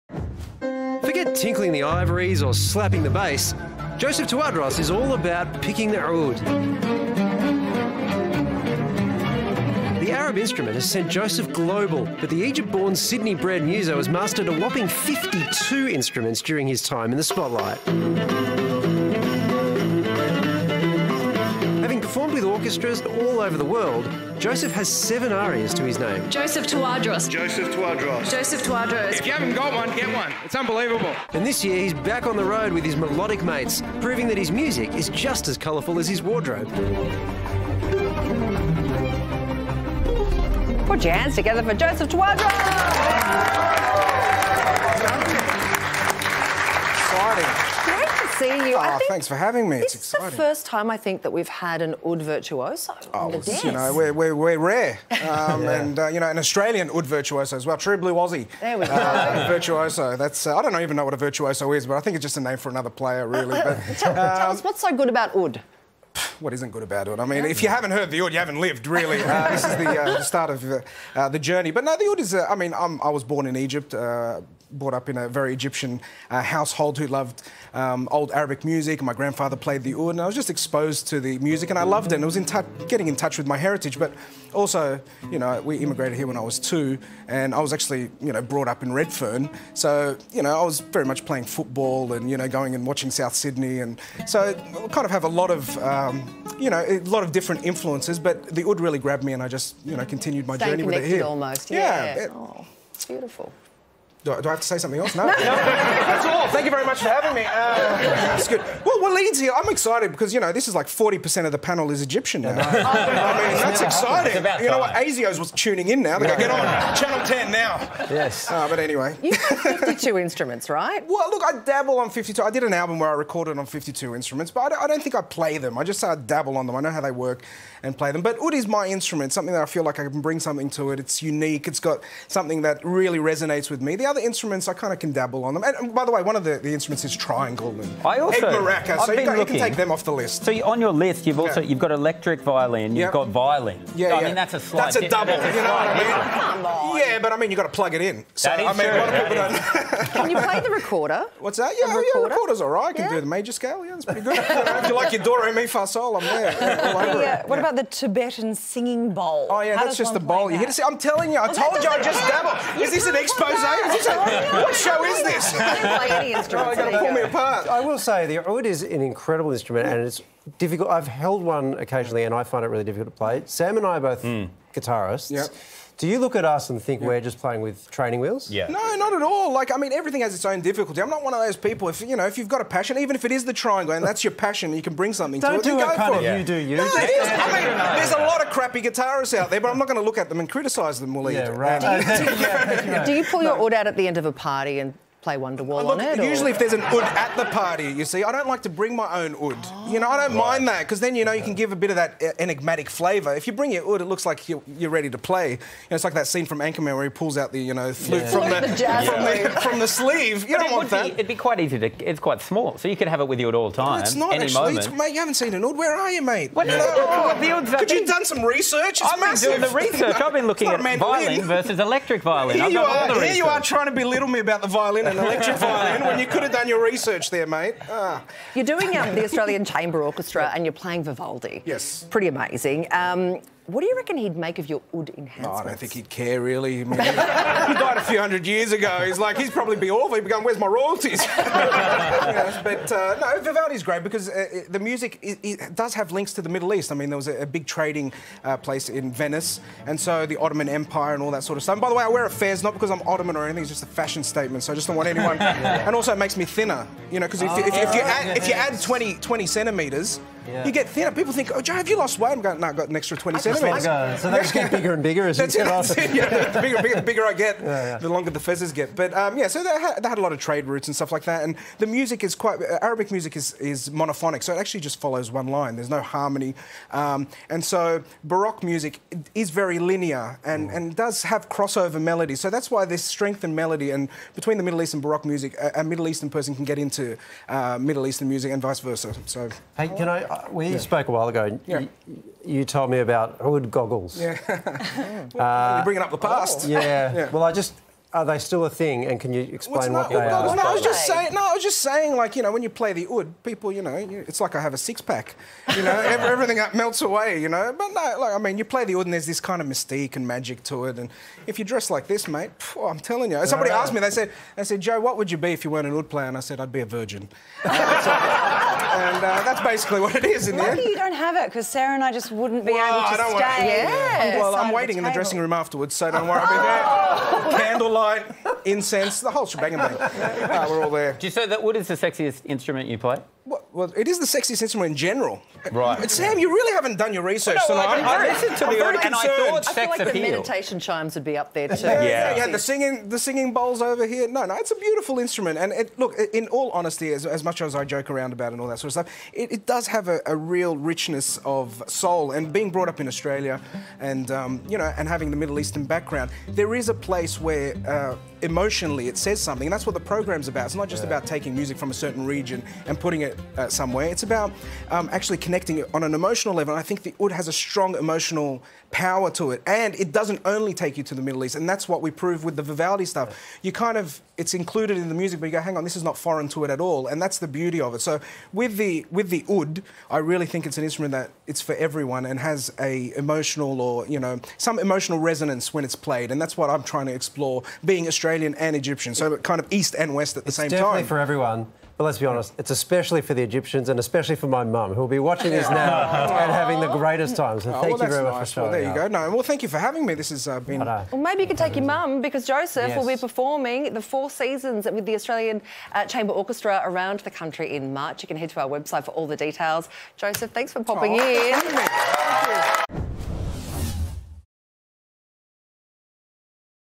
Joseph Tawadros: The Man Who Can Play 52 Instruments
Joseph Tawadros is best known for playing an oud but he can actually 'dabble' in 52 instruments, so, how many does he actually know how to play? We found out.